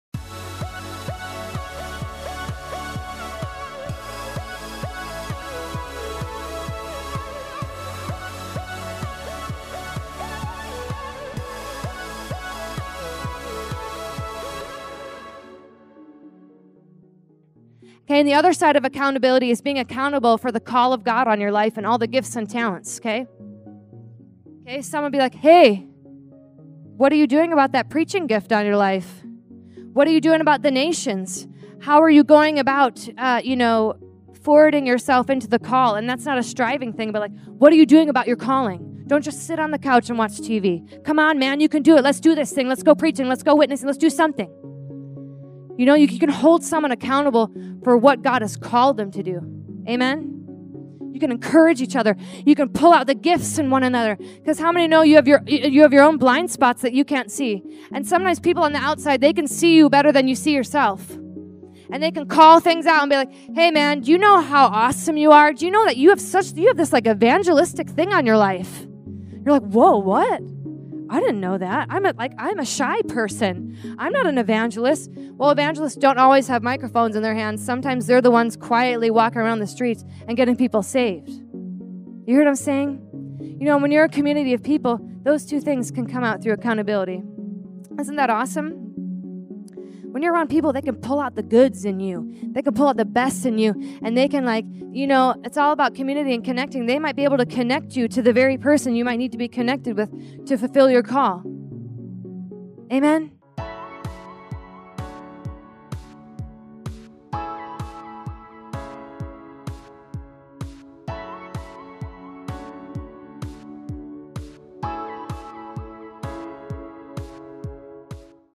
Speaking on Community - Pastor Rebekah Billings
Pastor Rebekah Billings preaches a message of importance of having a good community of people around you.

When two or more gather in Jesus' name, He shows up. So let's all get together summer 2016, love Jesus and fellowship with one another in our communities.